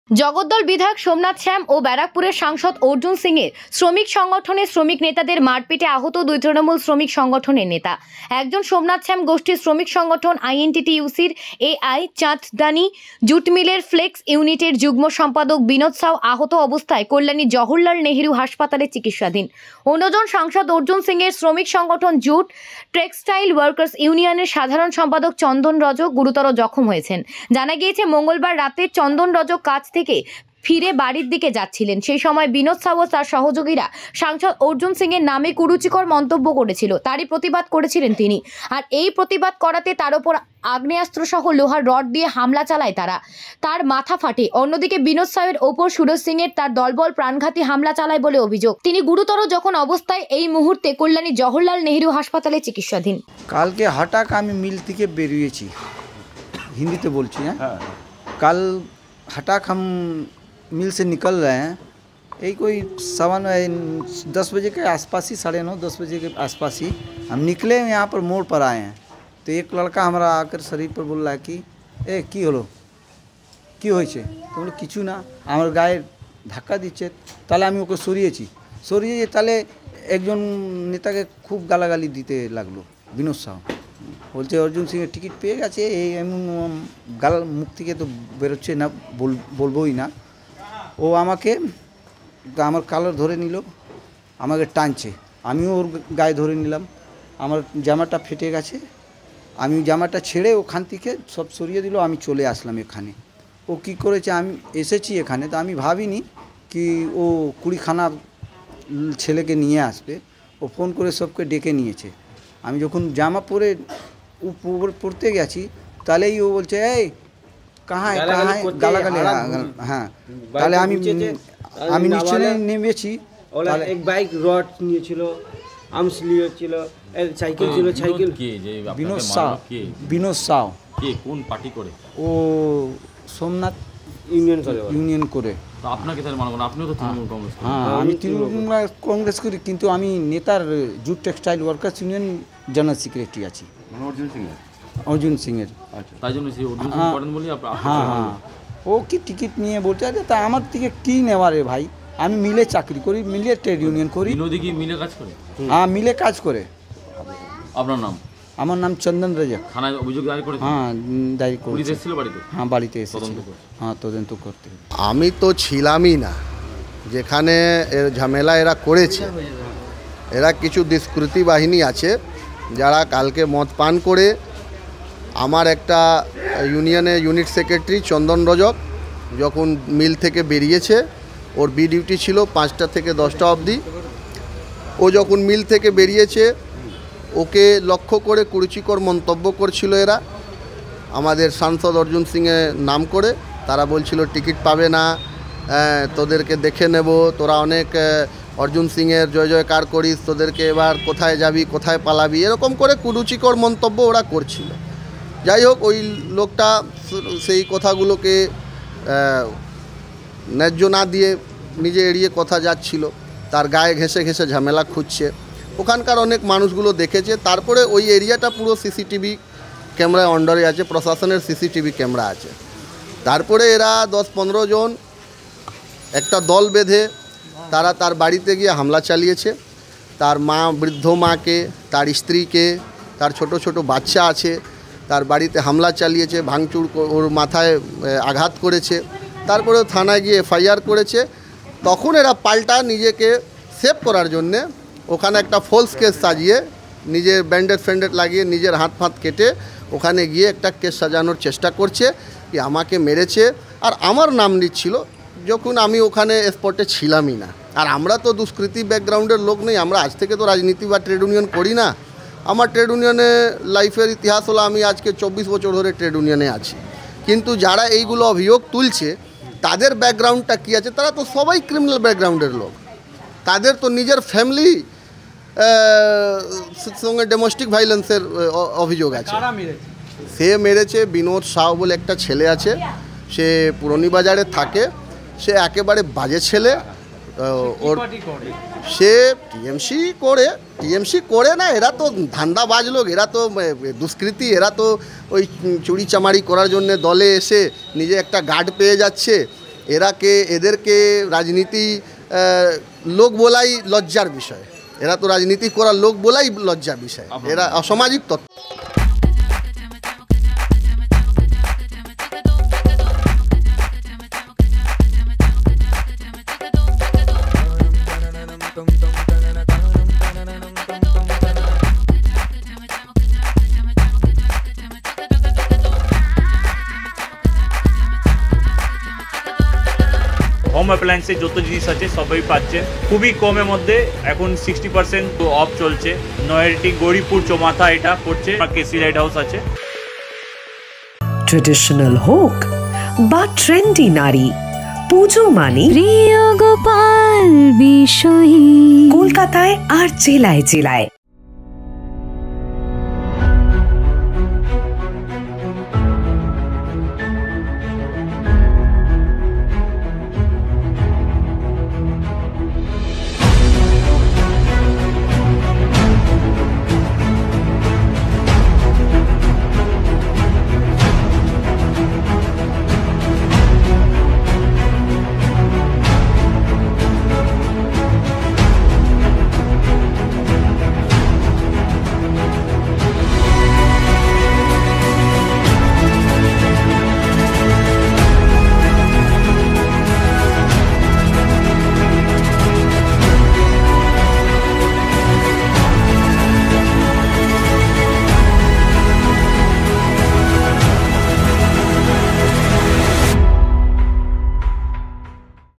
জগদ্দল বিধায়ক সোমনাথ শ্যাম ও ব্যারাকপুরের সাংসদ অর্জুন সিং এর শ্রমিক সংগঠনের শ্রমিক .....
জগদ্দল বিধায়ক সোমনাথ শ্যাম ও ব্যারাকপুরের সাংসদ অর্জুন সিং এর শ্রমিক সংগঠনের শ্রমিক নেতাদের মা*রপিটে আহ*ত দুই তৃণমূল শ্রমিক সংগঠনের নেতা।.